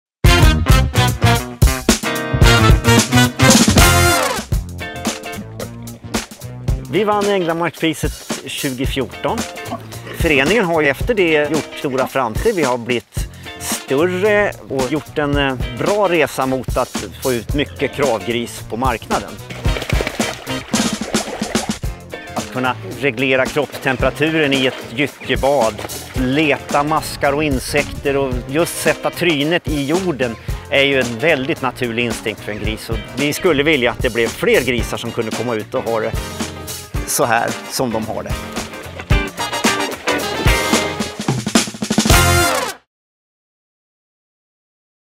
Nominera till Änglamarkspriset 2022
Nominera den person eller det företag som du tycker står för värdefulla insatser inom miljö och ekologi. Vinnaren får 250 000 kr. Nominera här: